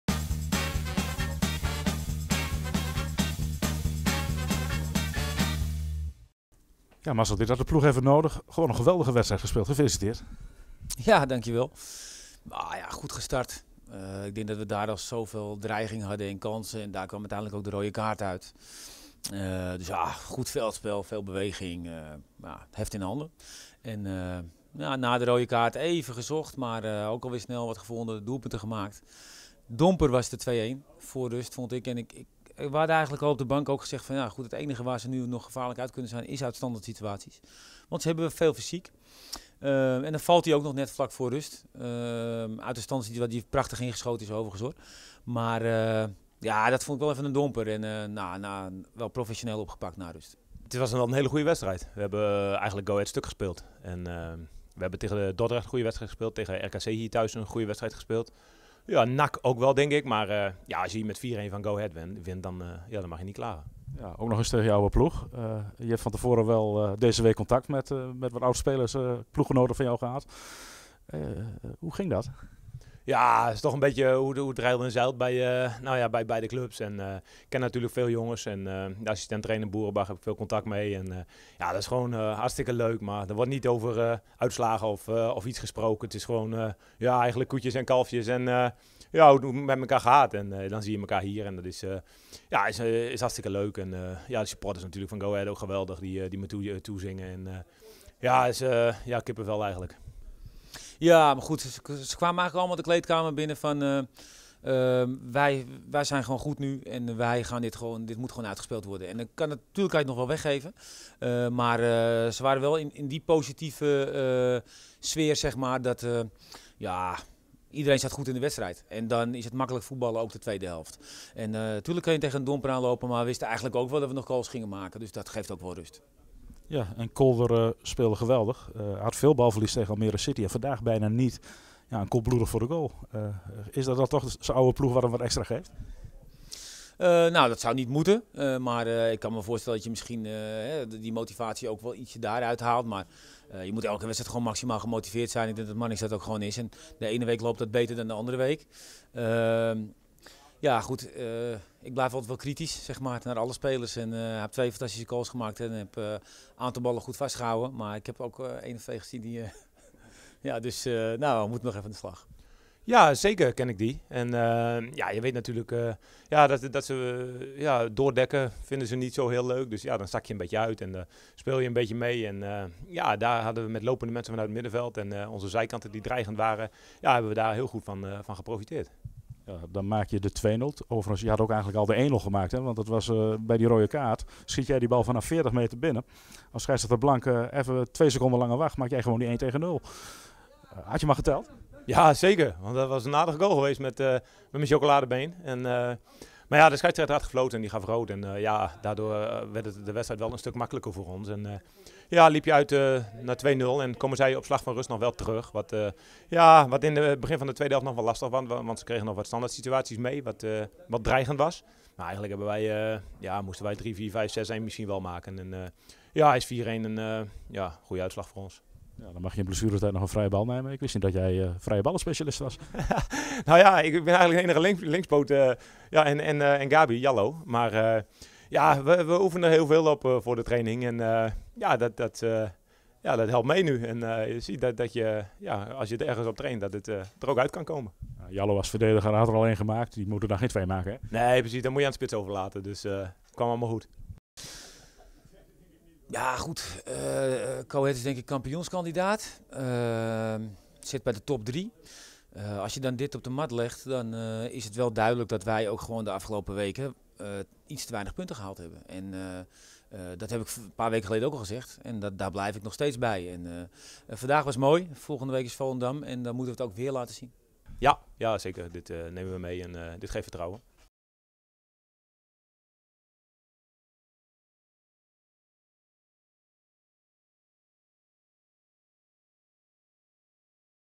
FC Emmen wint overtuigend van Go Ahead Eagles
FC Emmen heeft zich uitstekend hersteld van de 4-0 nederlaag tegen Almere City FC. In de JENS Vesting werd titelkandidaat Go Ahead Eagles met grote cijfers verslagen. Emmen won met 4-1 en  Marnix Kolder scoorde twee keer tegen zijn oude ploeg.